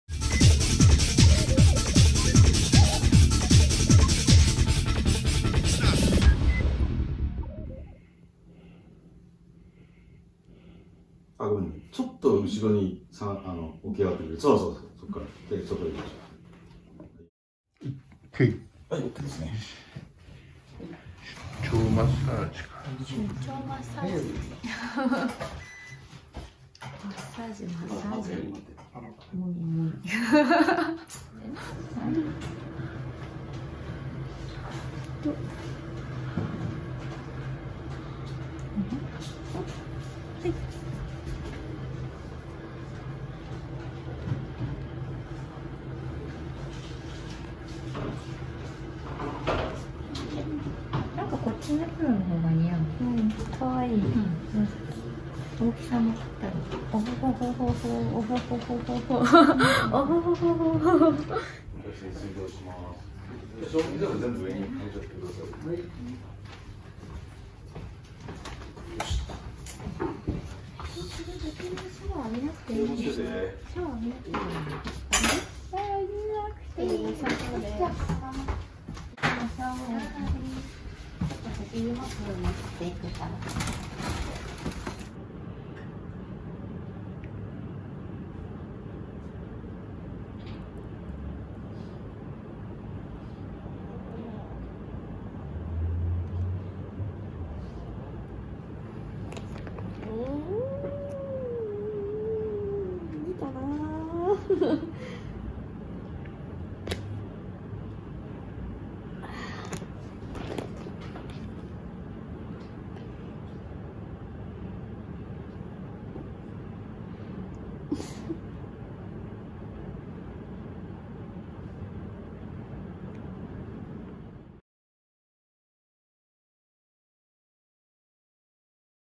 リカさん ドラマ撮影をこなして、しばしの間休憩中(//∇//) #4｜撮影現場生中継＆本格ドラマ作品 NAGIRAチャンネル
逢見リカさん主演ドラマ作品撮影舞台裏

リカさん ドラマ撮影をこなして、
しばしの間休憩中(//∇//)

次回につづく…

『ドラマ撮影の舞台裏、ドキュメンタリーを動画でアップ中！』